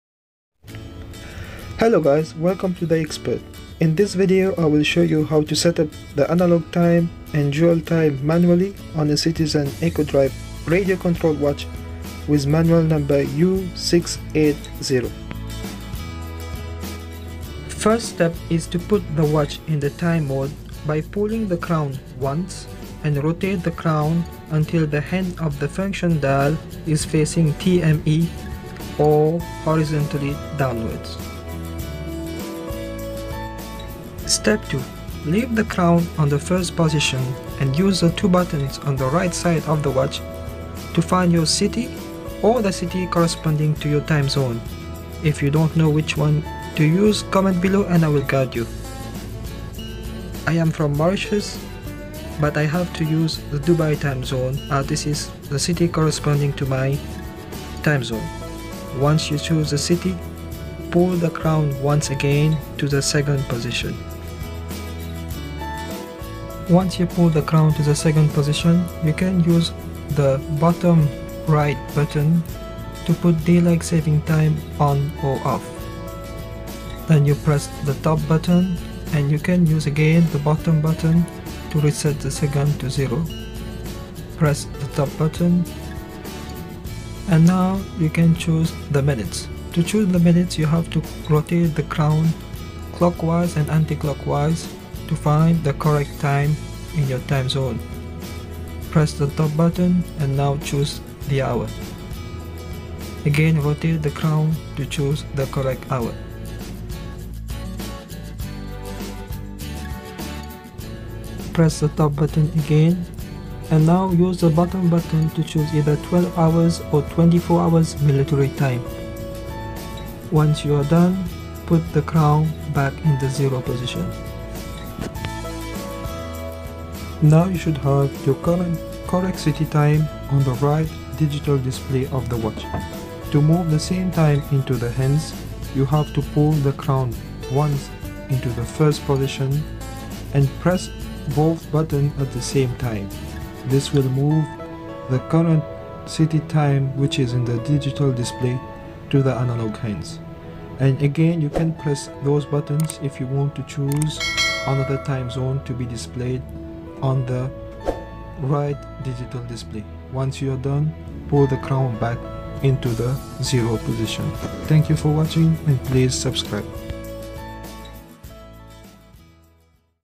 How to set Analog and Digital Dual Time Citizen Radio Controlled Navihawk Skyhawk (Caliber U68*)
Quick and easy tutorial video on how to set the analog hands and digital display time (dual time) manually on a citizen radio controlled atomic timekeeping watch wr200 Navihawk or Skyhawk.

Citizen Radio Controlled Navihawk Setting Instruction.

Step by step instructions on how to set up your Citizen radio controlled watch with manual number U68* or Caliber (Cal.) U680.

To set the date and dual time on Citizen models:
JY8078-52L
JY8078-O1L
JY8078
JY8035
JY8035-04E
JY8035-55E
JY8330
JY8330-83E
JY8075
JY8075-51E
JY8037
JY8037-50E
JY8020
JY8020-52E
JY8025
JY8025-59E
JY8034
JY8034-58E
JY8050-51E
JY8051-59E
JY8051-08E
JY8058-50L
JY8059-57E
JY8068-56E
JY8069-88E
JY8070-54E
JY8077-04H

This also works for the Blue Angel collection dual time watch.